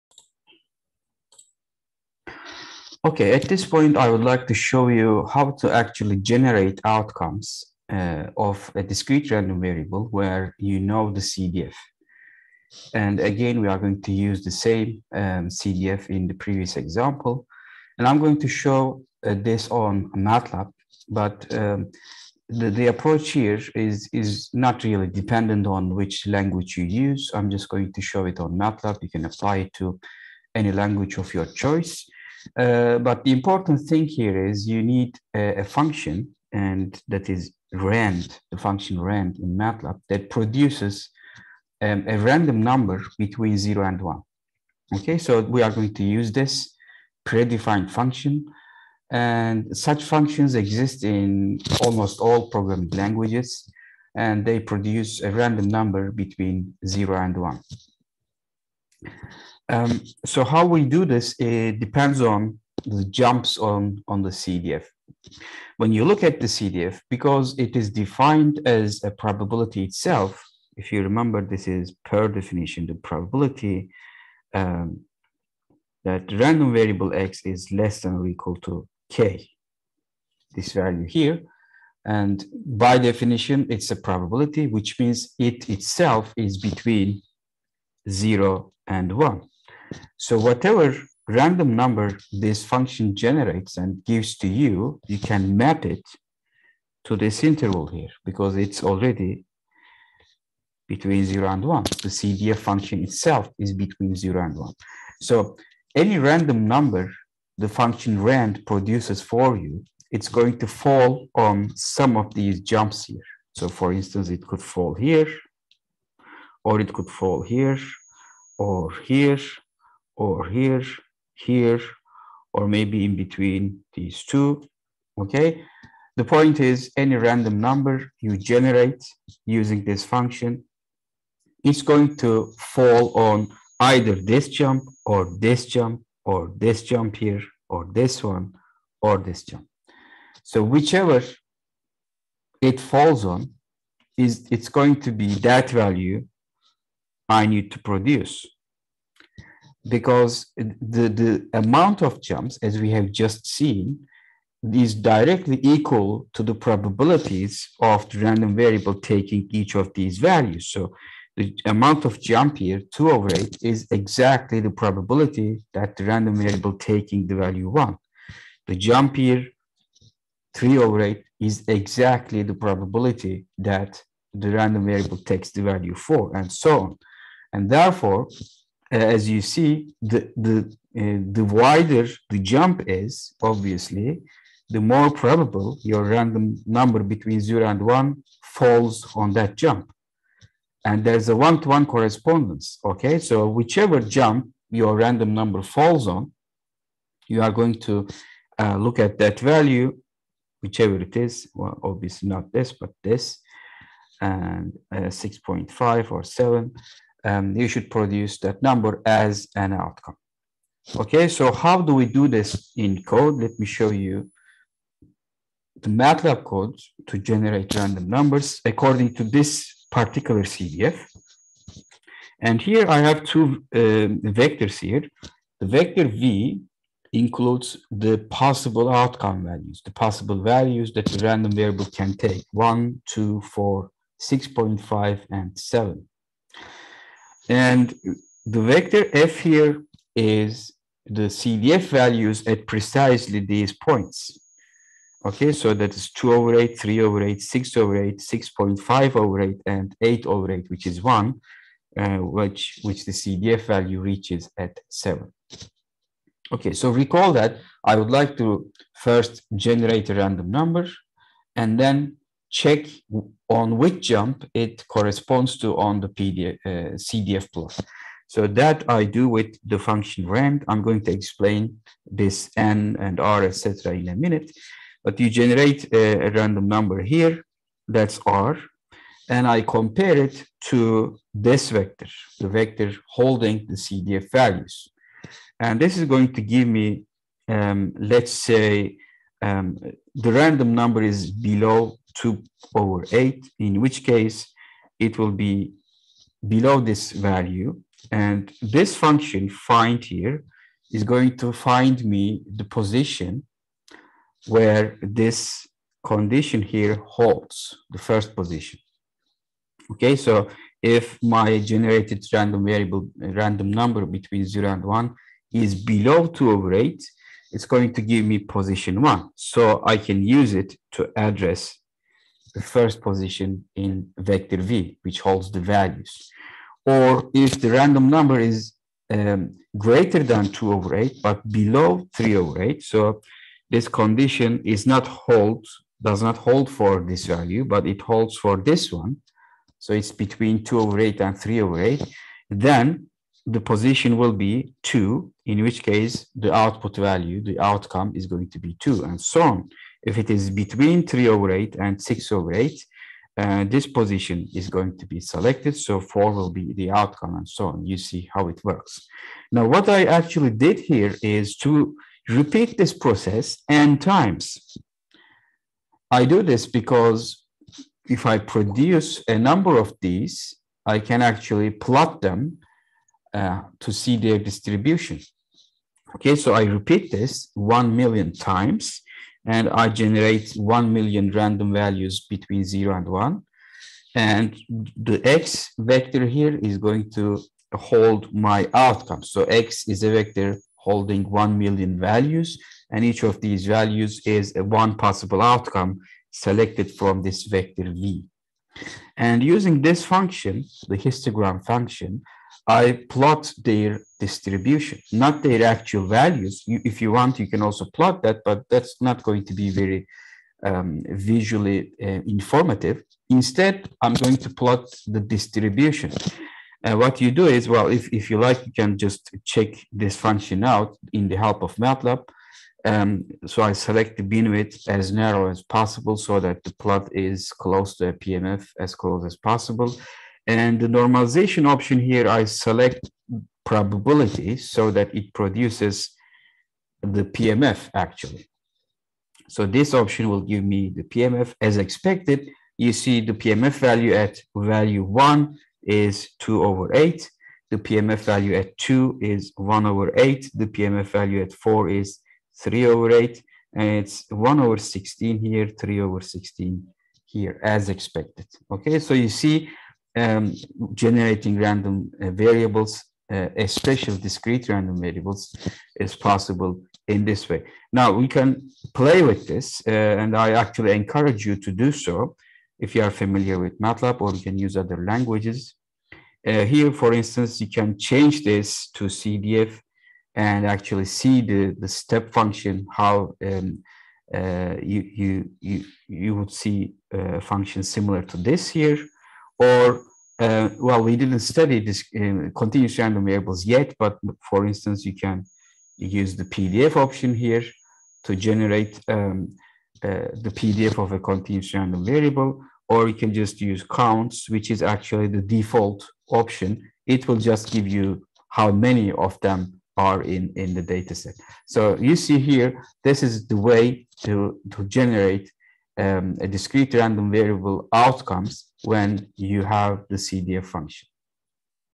03.08 Generating Discrete Random Variables
How do we generate a sample outcome of a discrete random variable with known cdf using Matlab? How can we visualize the distribution of a large number of such samples?
Probability & Stochastic Processes course at İstanbul Technical University.